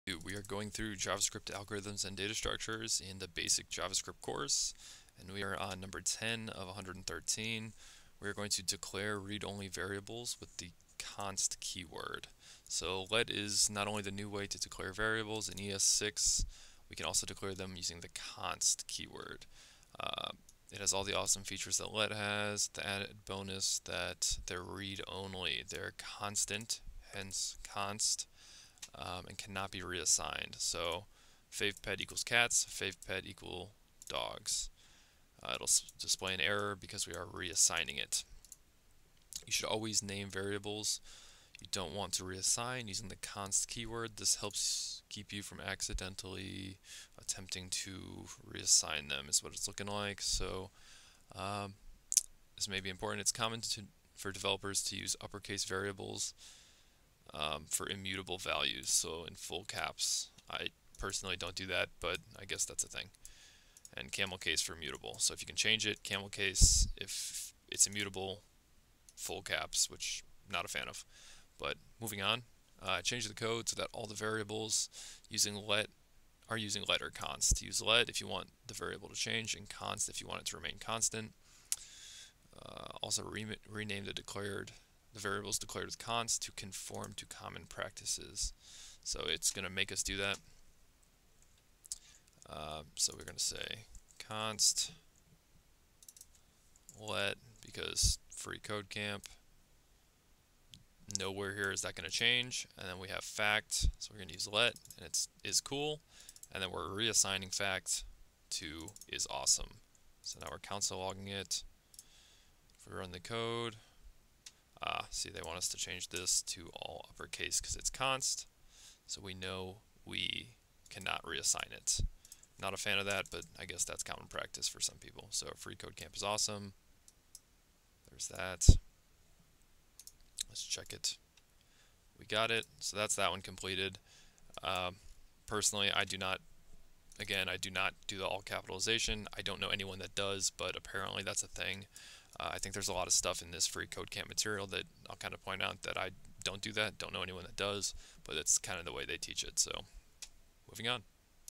Declare a Read-Only Variable with the const Keyword - Free Code Camp Help - Basic Javascript - FCC
Declare a Read-Only Variable with the const Keyword Challenge